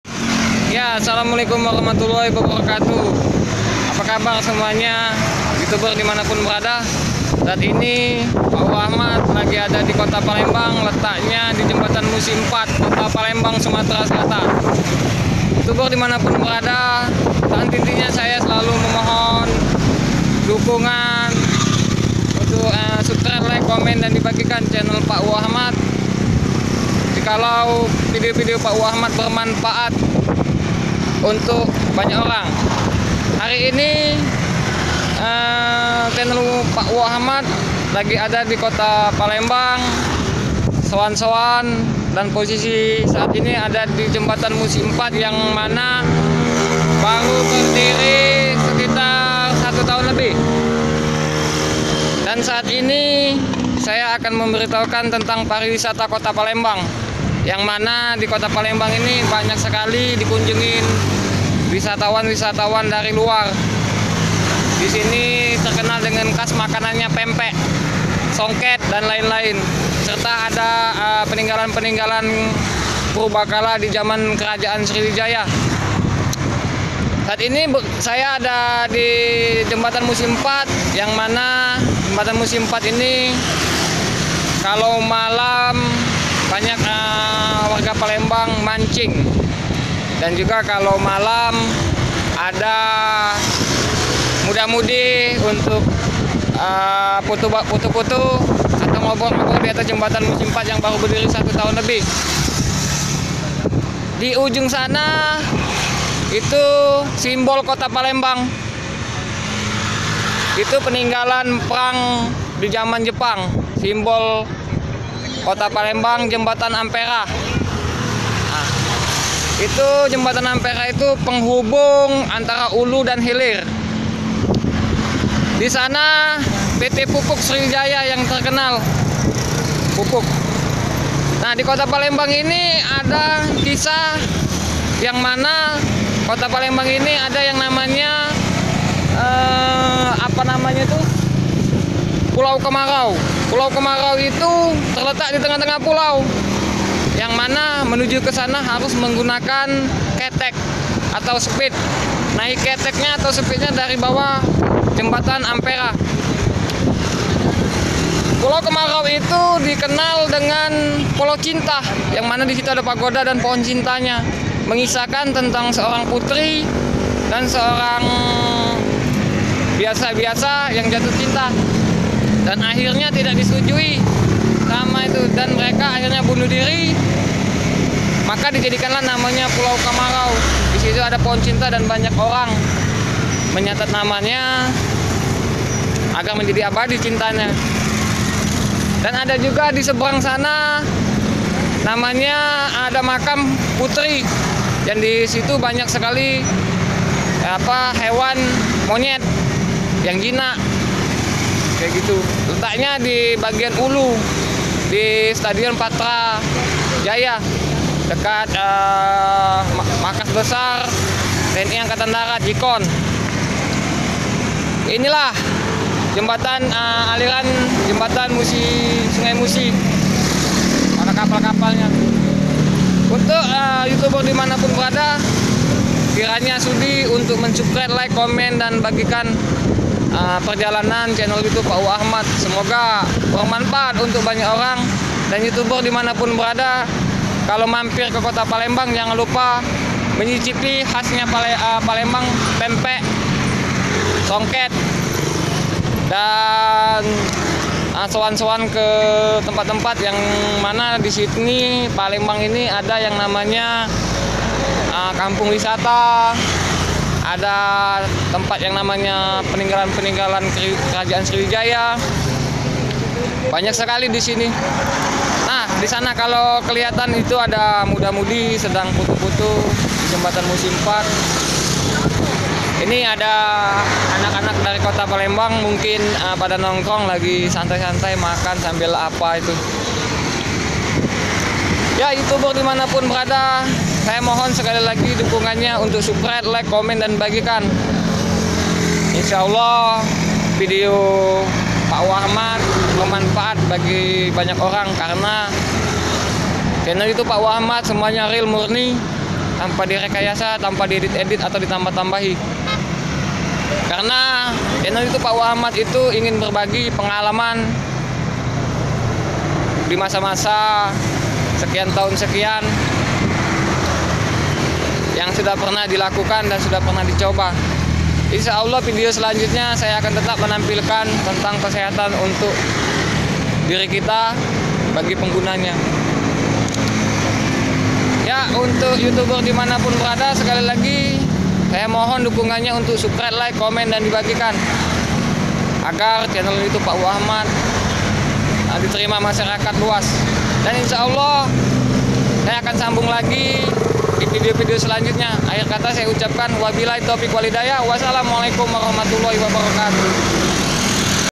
INDAH NYA KOTA PALEMBANG
KOTA PALEMBANG YG TERKENAL DGN KERAJAAN SRIWIJAYA DGN SIMBOL JEMBATAN AMPERA DAN CIRI KHAS MAKANAN NYA PEMPEK DAN PINDANG DLL YA BUAT YOUTUBER DIMANA PUN BERADA KIRA NYA SUDIH MENDUKUNG CHANNEL YOUTUBE NYA PAKWO AHMAD.